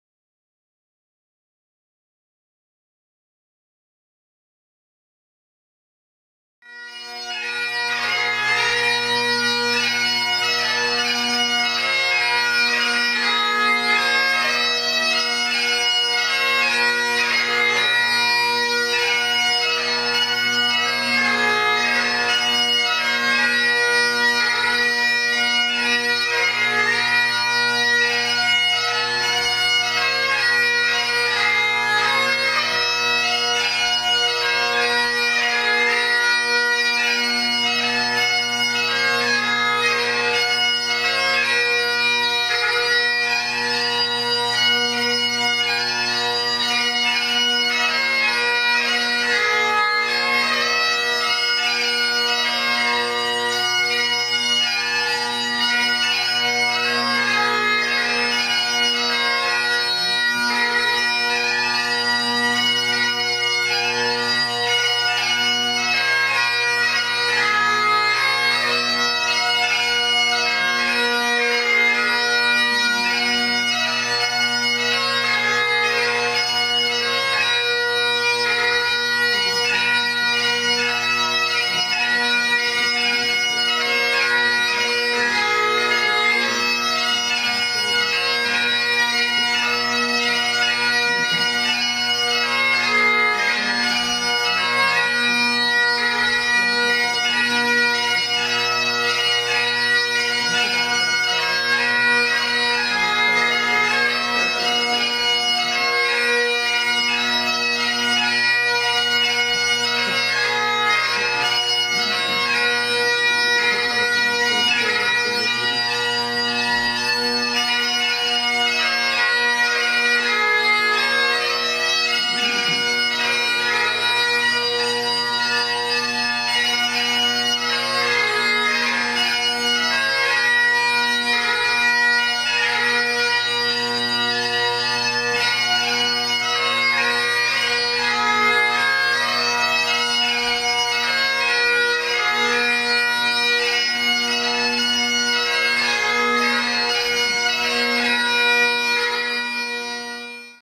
Ceol Na Pioba Solo Bagpipe Competition 2009 George Low 6/8 March
George Low competing in grade 4 sr. 6/8 March Saturday, October 24, 2009 at the Karpeles Manuscript Library Museum in Shreveport, Louisiana.